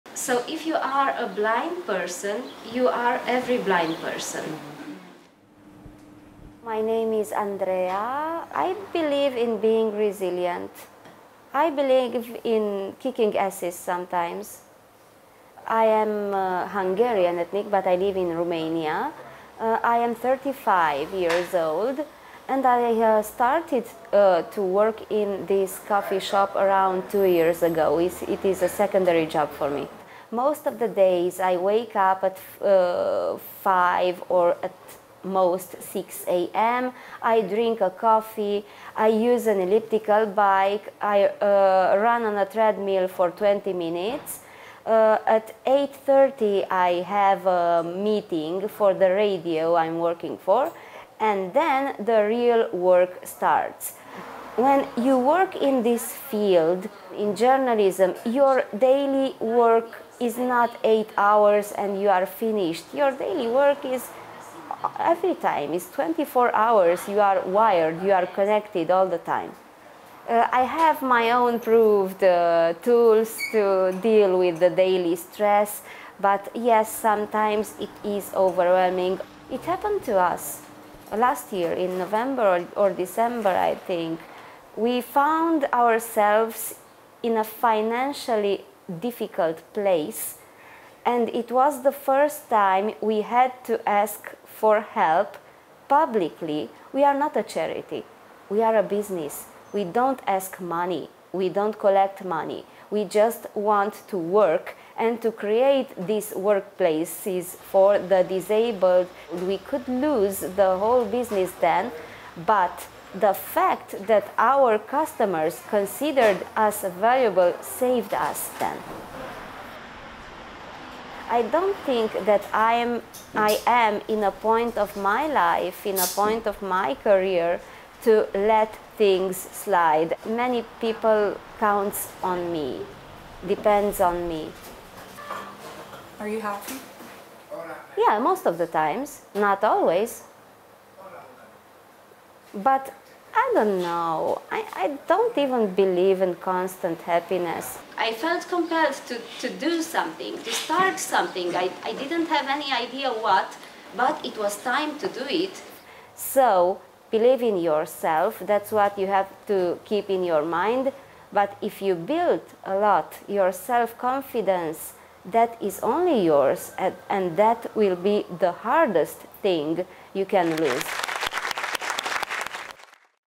Andrea Nagy: "I believe in being resilient"
Andrea Nagy (35) is a journalist and does marketing for a social coffee shop in Cluj-Napoca, Romania. She is blind and hosts a popular radioshow.

Authors: Teun Eijkelboom, Mihael Fitz, Stiina Hekkala, Keti Machavariani, Ilinca Pleș
Mentor: Guillaume Moulaert

Project: Invisible Cities, May 2024